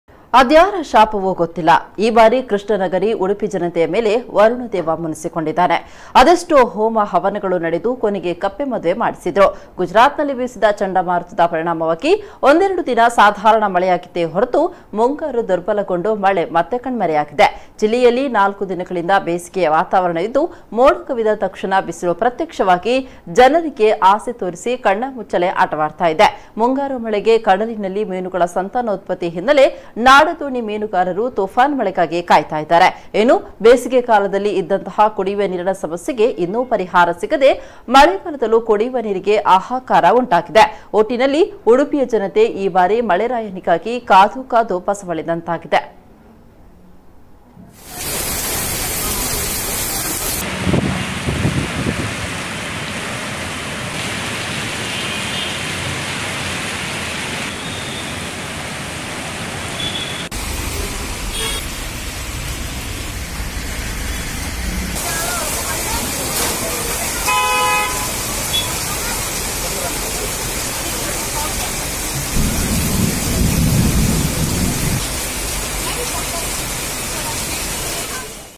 ಉಡುಪಿ ಜಿಲ್ಲೆಯಲ್ಲಿ ಕಣ್ಮರೆ ಯಾದ ವರುಣ
ಅದ್ಯಾರ ಶಾಪವೋ ಗೊತ್ತಿಲ್ಲ, ಈ ಬಾರಿ ಕೃಷ್ಣನಗರಿ ಉಡುಪಿ ಜನತೆಯ ಮೇಲೆ ವರುಣ ದೇವ ಮುನಿಸಿಕೊಂಡಿದ್ದಾನೆ. ಅದೆಷ್ಟೋ ಹೋಮ ಹವನಗಳು ನಡೆದು, ಕೊನೆಗೆ ಕಪ್ಪೆ ಮದುವೆ ಮಾಡಿಸಿದರು.. ಗುಜರಾತ್‍ನಲ್ಲಿ ಬೀಸಿದ ಚಂಡಮಾರುತದ ಪರಿಣಾಮ ಒಂದೆರಡು ದಿನ ಸಾಧಾರಣ ಮಳೆಯಾಗಿತ್ತೇ ಹೊರತು, ಮುಂಗಾರು ದುರ್ಬಲಗೊಂಡು ಮಳೆ ಮತ್ತೆ ಕಣ್ಮರೆಯಾಗಿದೆ